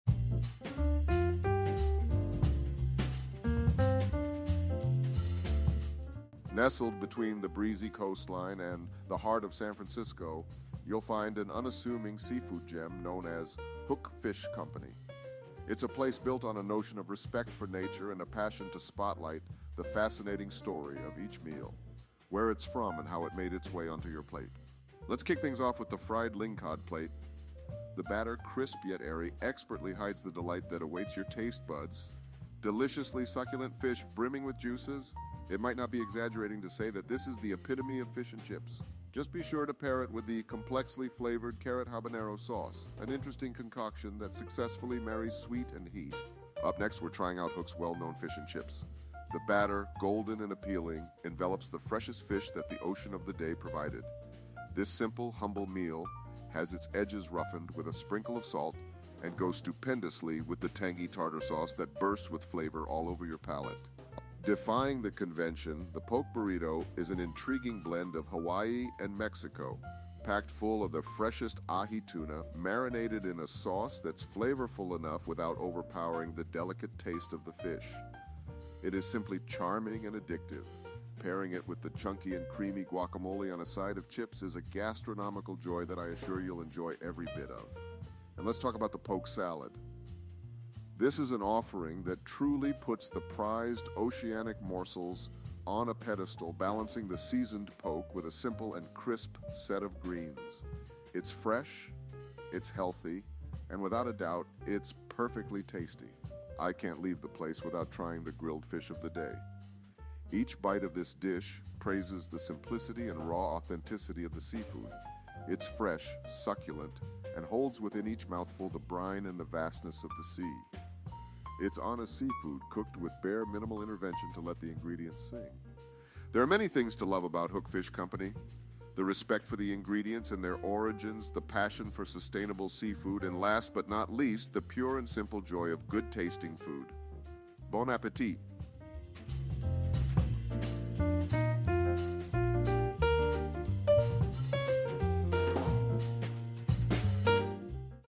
Fried Lingcod Plate | Hook Fish | San Francisco, CA
Based on the reviews, menu, and image captions, I'm going to generate a recipe for the Fried Lingcod Plate, a dish that many visitors seem to enjoy.

Recipe for Fried Lingcod Plate:

Ingredients:
For the fish:
- 4 lingcod fillets, about 6 ounces each
- Salt and pepper, to taste
- 1 1/2 cups all-purpose flour
- 1 cup beer
- 1 large egg

For the Carrot Habanero Sauce:
- 3 Habanero peppers
- 2 Carrots
- 1 Onion
- 2 cloves Garlic
- 1 cup White vinegar

Instructions:
1.  To make the Carrot Habanero Sauce, peel carrots and chop finely.  Dice the onion, garlic, and habaneros (be careful with these).
2.  Put all ingredients in a pot, add vinegar, and simmer until carrots are soft and tender.  Let it cool down a bit and then puree it in a blender until smooth.  Salt to taste.
3.  Preheat your oil in a deep fryer or large pot to 375 degrees F (190 degrees C).
4.  Season the lingcod fillets with salt and pepper.
5.  In a medium bowl, combine the flour, beer, and egg to create the batter.  Stir until smooth.
6.  Dip each fillet into the batter, letting the excess drip off, then carefully place in the preheated oil.
7.  Fry the fish, turning once, until golden brown and cooked throughout, about 4 to 5 minutes per side.  Adjust the heat as necessary to maintain the oil temperature.
8.  Use a slotted spoon to remove the fish from the oil, draining it on a plate lined with paper towels.
9.  Serve your Fried Lingcod immediately, paired with the Carrot Habanero Sauce on the side.

Enjoy your Fried Lingcod Plate from Hook Fish Co at home!